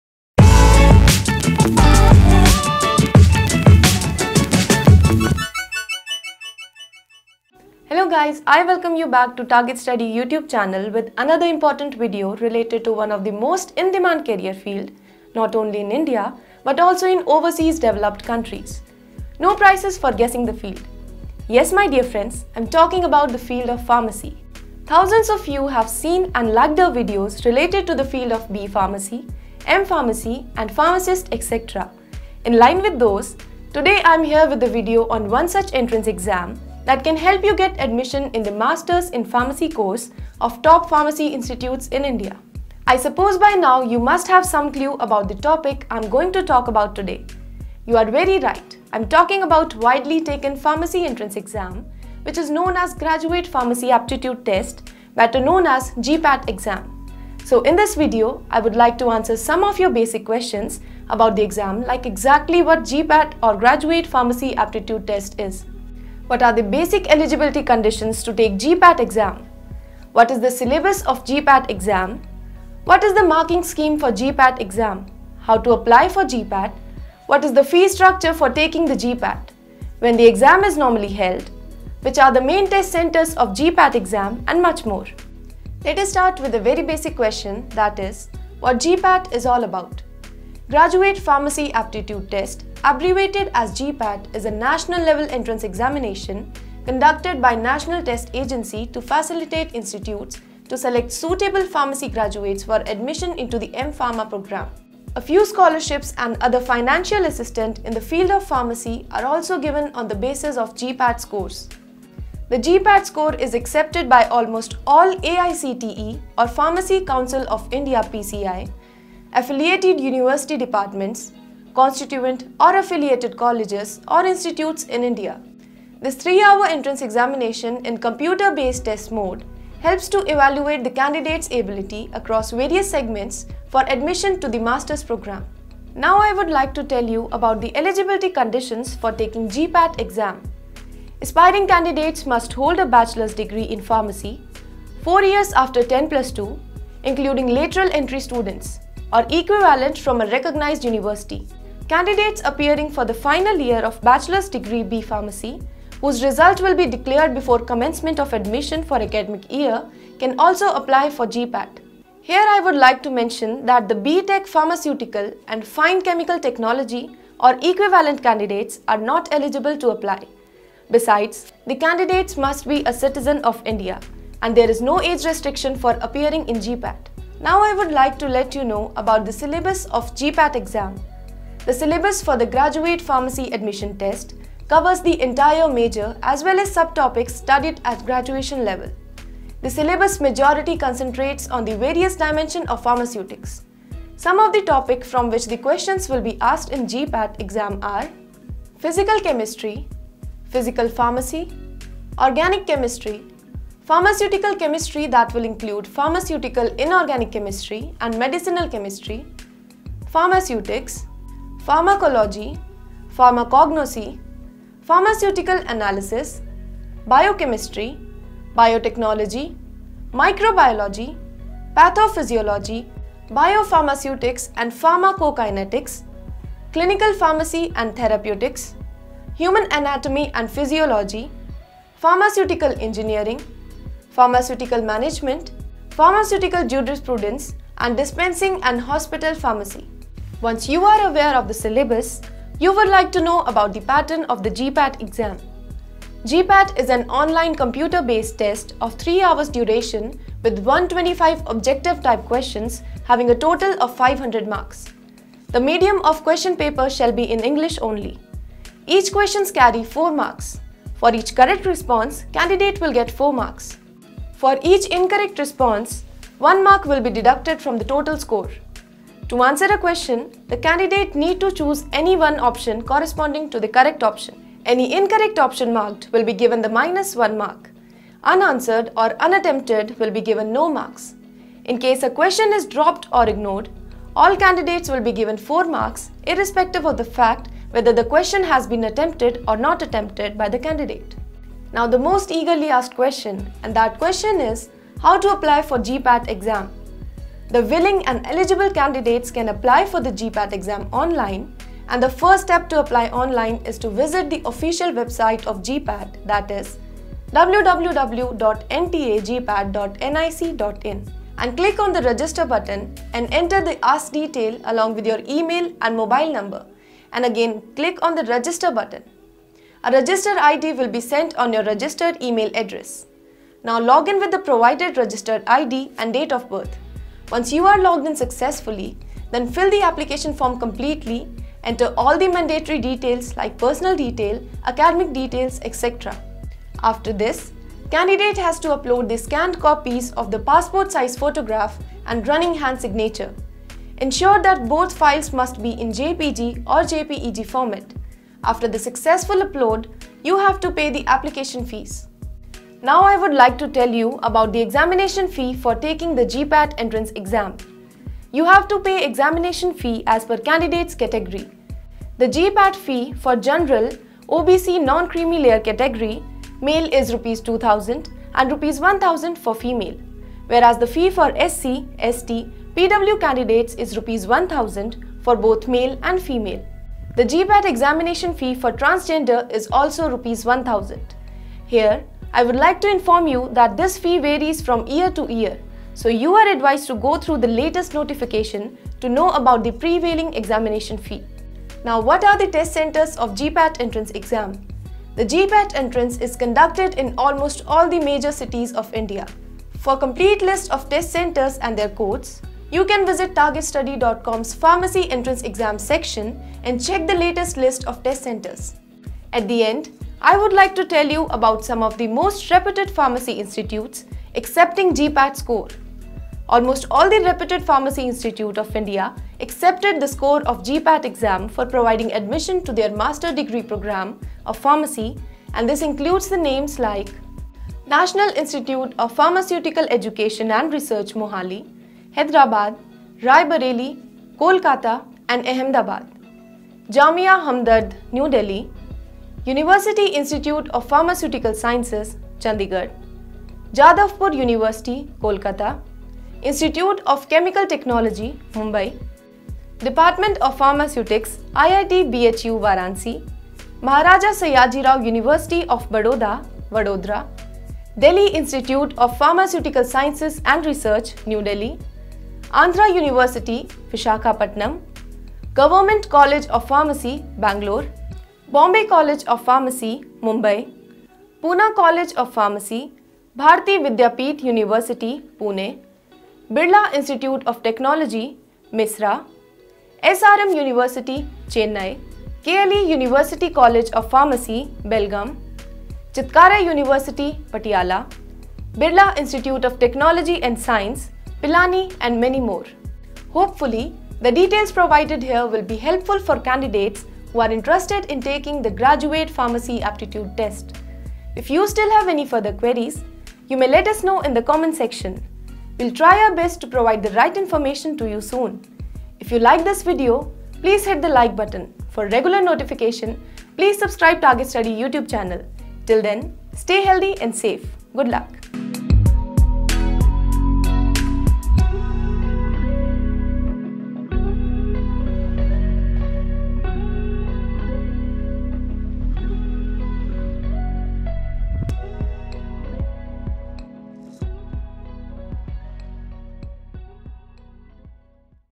GPAT - Graduate Pharmacy Aptitude Test | What is GPAT? | Syllabus | Pattern | Fee
In this video, I would like to answer some of your basic questions about the exam like what exactly GPAT or Graduate Pharmacy Aptitude Test is? What are the basic Eligibility conditions to take GPAT Exam? What is the syllabus of GPAT exam? What is the paper pattern for GPAT Exam? What is the marking scheme for GPAT Exam? How to apply for GPAT? What is the fee structure for taking the GPAT? When the exam is normally held?
Which are the main test centers of GPAT exam and much more?